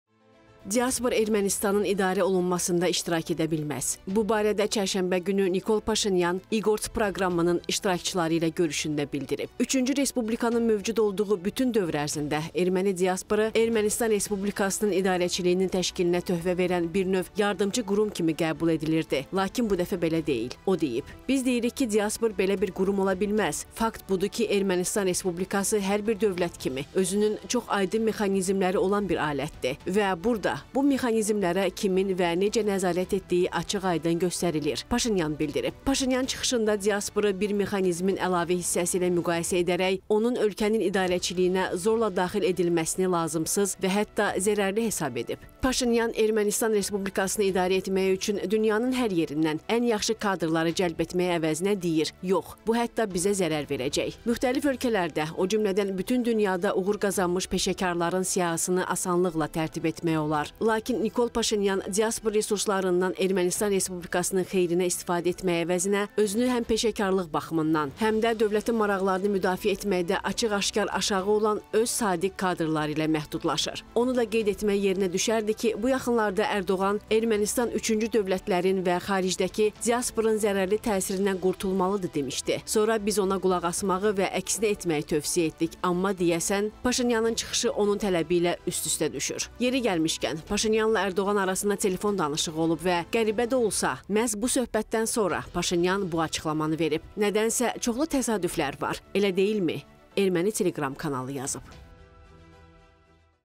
Paşinyana dedik ki, tərsini et, amma… | Erməni diasporu barədə qalmaqallı açıqlama
“Diaspor Ermənistanın idarə olunmasında iştirak edə bilməz”.
Bu barədə çərşənbə günü Nikol Paşinyan iGorts proqramının iştirakçıları ilə görüşündə bildirib.
“Üçüncü Respublikanın mövcud olduğu bütün dövr ərzində erməni diasporu Ermənistan Respublikasının idarəçiliyinin təşkilinə töhfə verən bir növ yardımçı qurum kimi qəbul edilirdi, lakin bu belə deyil”, – o deyib.
“Biz deyirik ki, diaspor belə bir qurum ola bilməz. Fakt budur ki, Ermənistan Respublikası hər bir dövlət kimi, özünün çox aydın mexanizmləri olan bir alətdir və burada bu mexanizmlərə kimin və necə nəzarət etdiyi açıq-aydın göstərilir”, – Paşinyan bildirib.
Paşinyan çıxışında diasporu bir mexanizmin əlavə hissəsi ilə müqayisə edərək, onun ölkənin idarəçiliyinə zorla daxil edilməsini lazımsız və hətta zərərli hesab edib.
“Paşinyan Ermənistan Respublikasını idarə etmək üçün dünyanın hər yerindən ən yaxşı kadrları cəlb etmək əvəzinə deyir: yox, bu, hətta bizə zərər verəcək. Müxtəlif ölkələrdə, o cümlədən bütün dünyada uğur qazanmış peşəkarların siyahısını asanlıqla tərtib etmək olar. Lakin Nikol Paşinyan diaspor resursundan Ermənistan Respublikasının xeyrinə istifadə etmək əvəzinə özünü həm peşəkarlıq baxımından, həm də dövlətin maraqlarını müdafiə etməkdə açıq-aşkar aşağı olan öz sadiq kadrları ilə məhdudlaşır.
Onu da qeyd etmək yerinə düşərdi ki, bu yaxınlarda Ərdoğan “Ermənistan üçüncü dövlətlərin və xaricdəki diasporunun zərərli təsirindən qurtulmalıdır” demişdi. Sonra biz ona qulaq asmağı və əksini etməyi tövsiyə etdik, amma deyəsən... Paşinyanın çıxışı onun tələbi ilə üst-üstə düşür.
Yeri gəlmişkən, Paşinyanla Ərdoğan arasında telefon danışığı olub və qəribə də olsa, məhz bu söhbətdən sonra Paşinyan bu açıqlamanı verib.
Nədənsə çoxlu təsadüflər var, elə deyilmi?”, – erməni telegram kanalı yazıb.